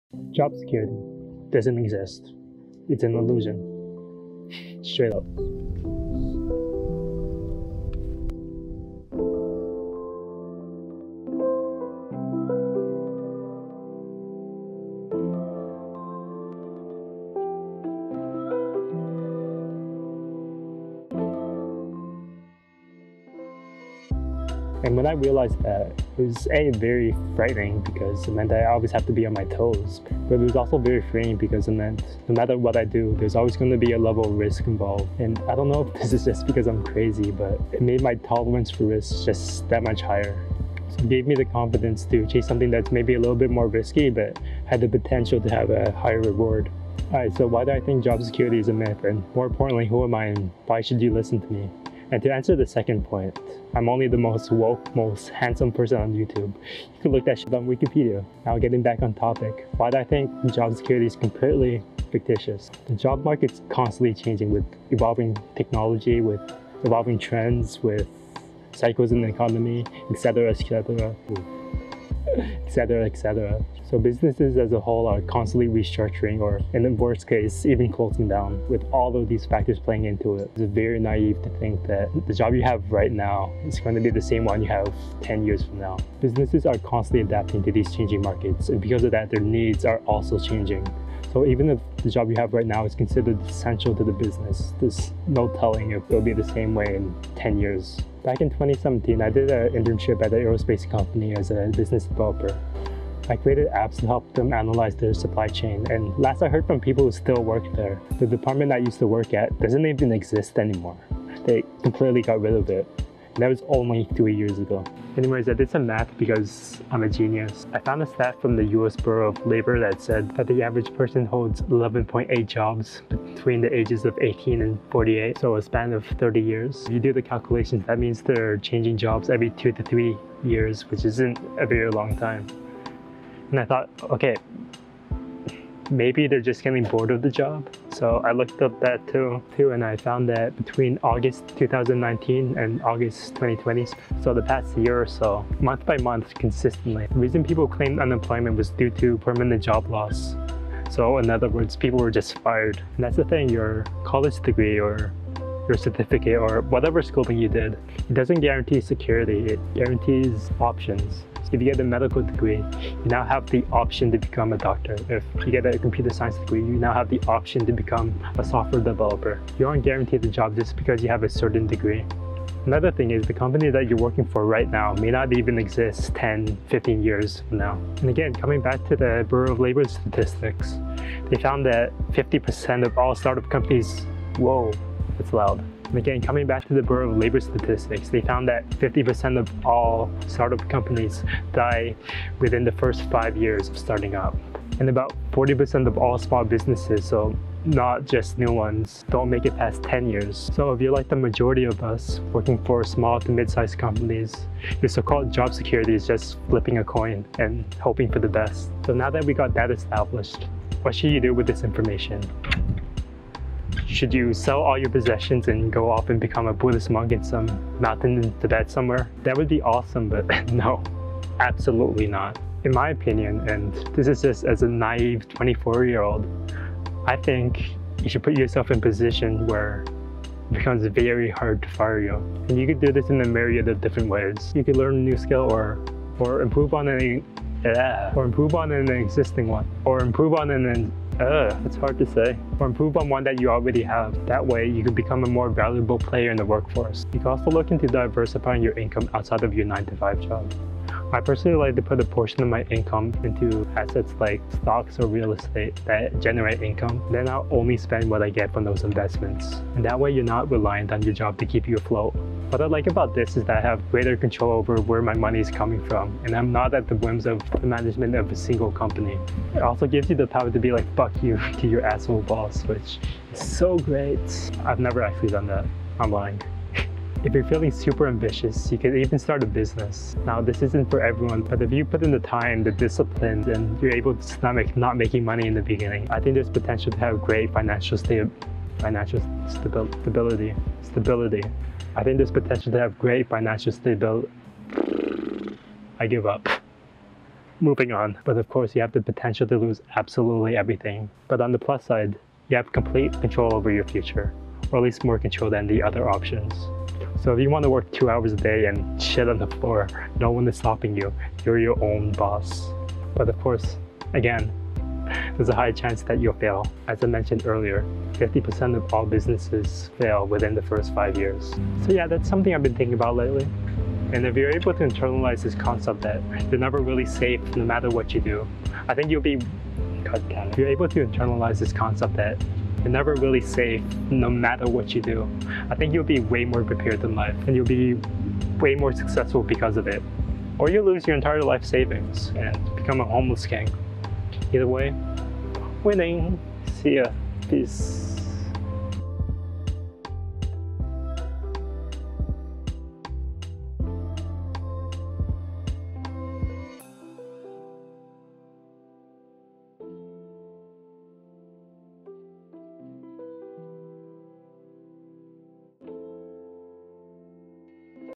Job Security Is A Myth (And What To Do About It)
Talking about why I think job security is completly fictitious and what to do about it to feel safe and be able to sleep well at night. Just my two cents as a naive 24 year old. I still have much to learn, young padawan.